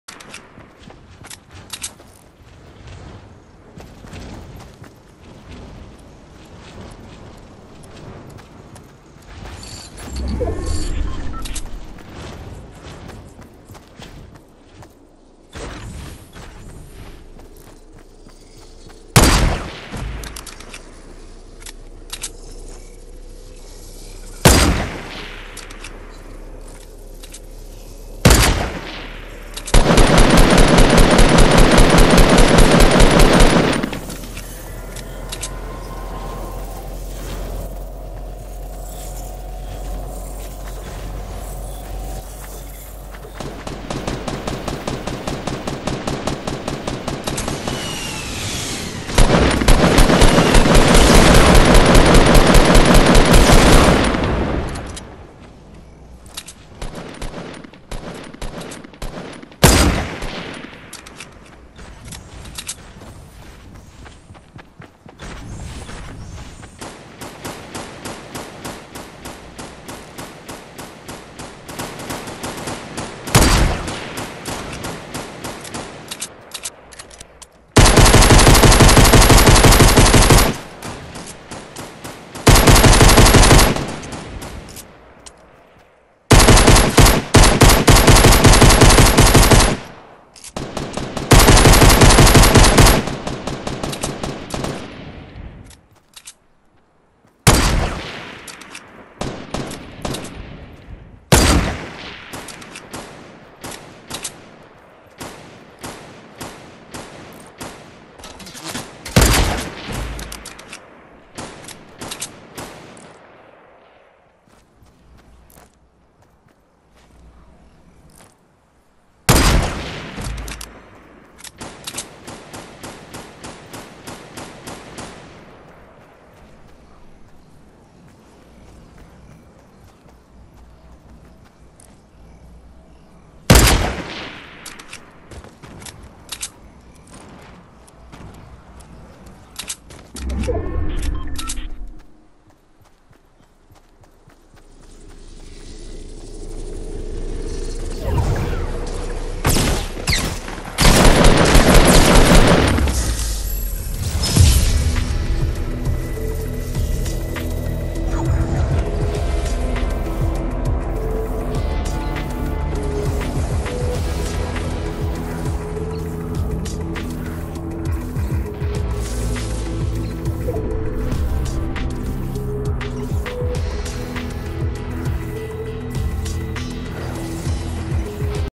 Epic showdown
Getting some epic snipes. Taking out 5 guys at the end and the last guy who was camping in our partners base Scaring the shit out of me gets turned on.